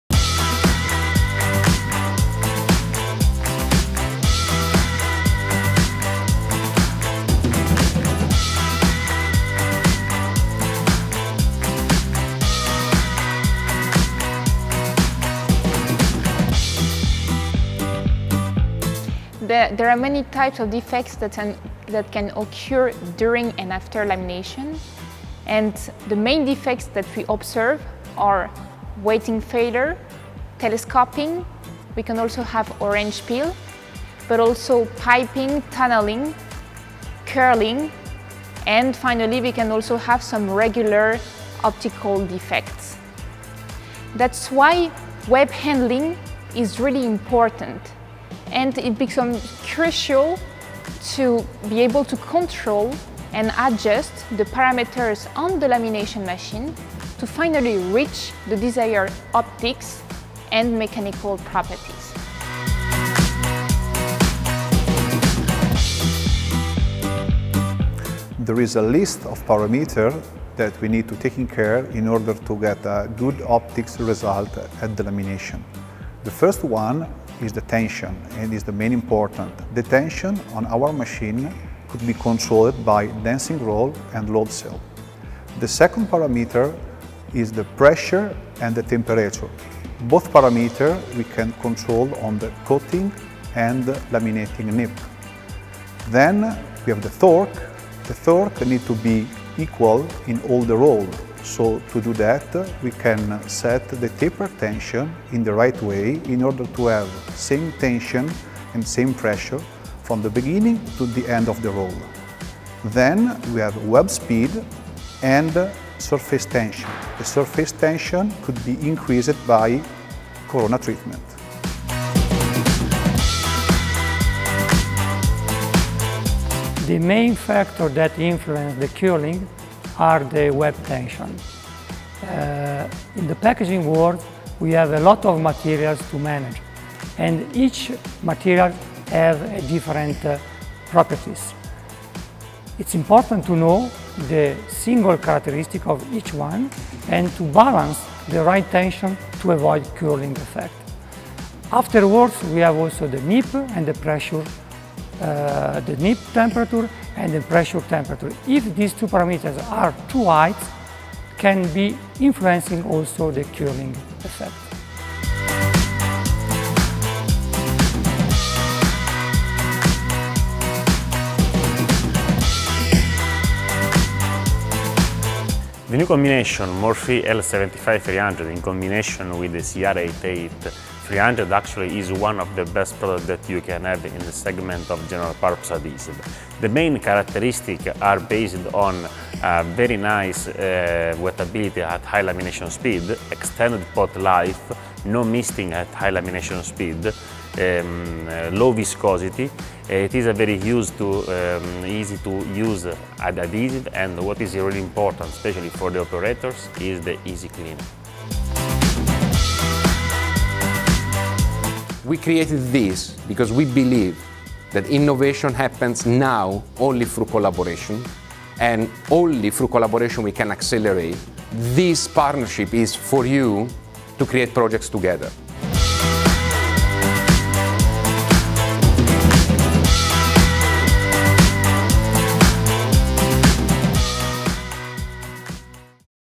Dow Pack Studios Academy: Lamination Insights Shared By Adhesives Experts
In this short web handling and troubleshooting video, Dow and Nordmeccanica experts share valuable insights on what defects can occur during lamination at a lamination line and how to deal with these for all types of lamination machines. In addition, learn more about the advantages of using Dow MOR-FREE™ L75-300 with the new coreactant CR 88-300.